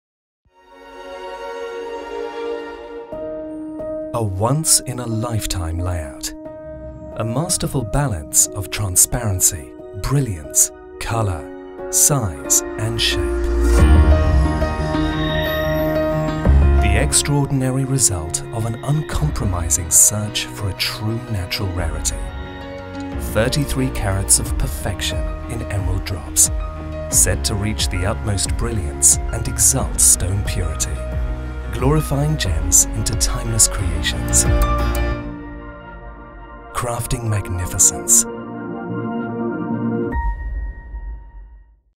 Acanthus Emerald necklace | Mediterranea High Jewelry Collection
The craftsmanship of the Acanthus Emerald necklace beautifully showcases the intricate artistry and meticulous attention to detail that goes into creating our exquisite High Jewelry piece, capturing the essence of our master artisans' skill and expertise. Acanthus flowers decorated some of Antiquity's finest reliefs, swirling so vividly around the face of the Ara Pacis in Rome, here embelished with exceptional emeralds.